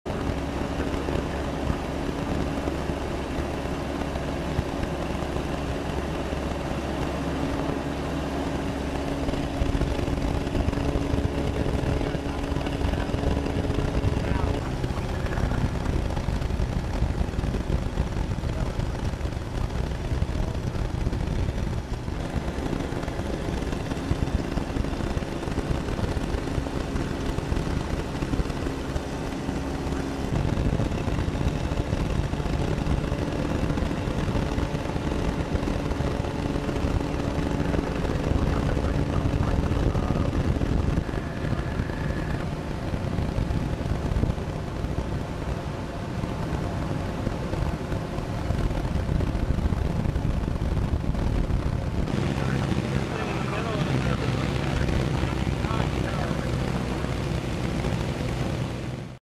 Raw Video: Winfield, Missouri Levee Breach
PlusRaw Video: Winfield, Missouri Levee BreachRaw Video: Winfield, Missouri Levee BreachThe Associated PressResidents in and around Winfield, Missouri were urged to evacuate their homes after another levee breach.  Authorities say a 280-mile stretch of the Mississippi River near Winfield is expected to remain closed for over a week. (June 19)This video contains ONLY natural sound. No script is available.